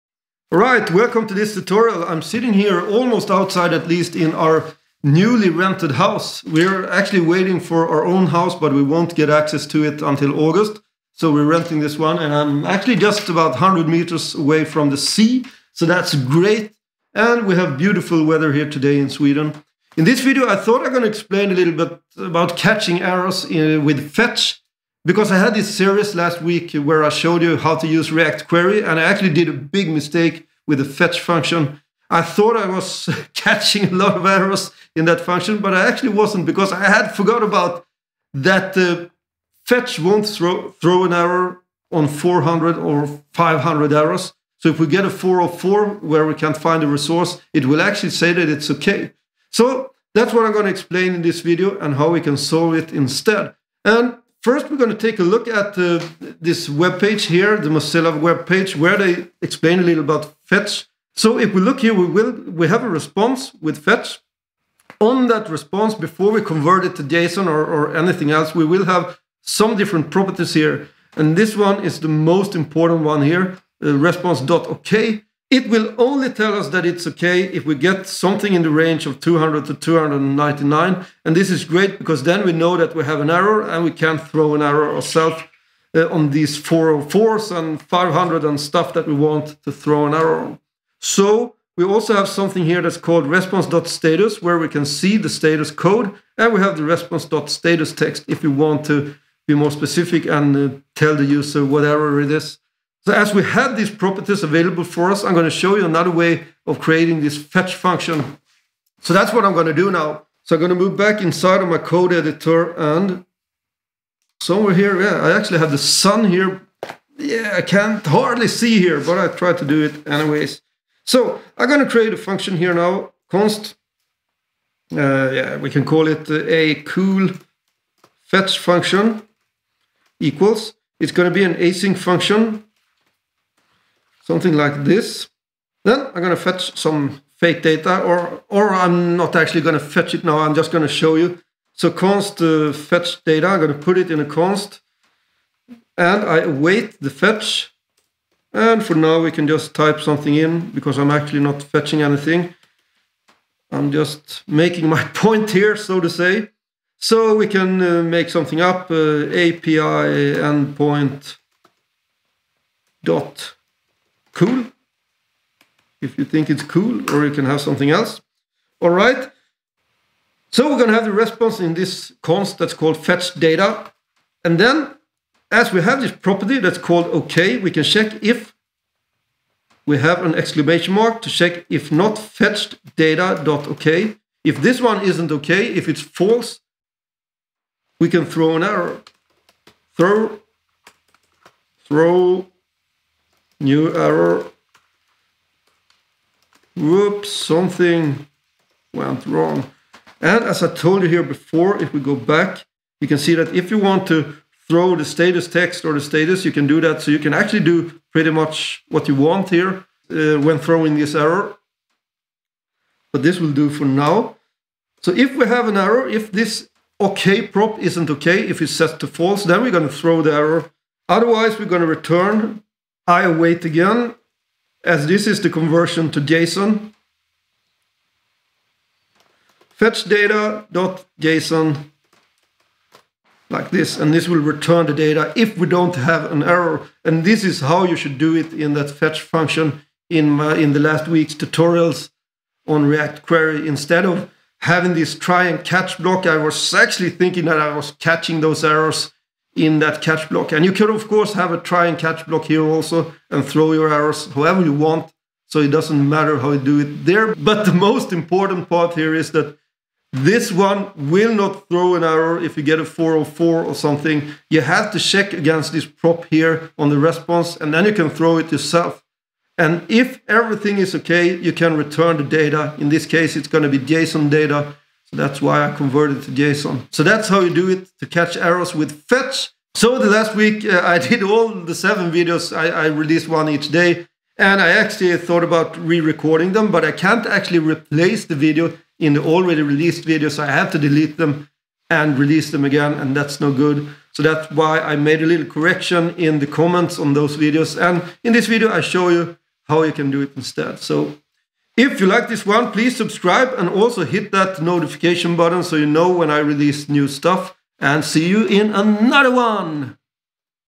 Catch errors with Fetch - Javascript
I'll show you how to handle response errors with Javascript fetch. This is also a correction to the react-query week videos where I did a mistake with the fetch function.

Do you want to master react, gatsby, or svelte fast? ⏩ 🚀 👩‍🚀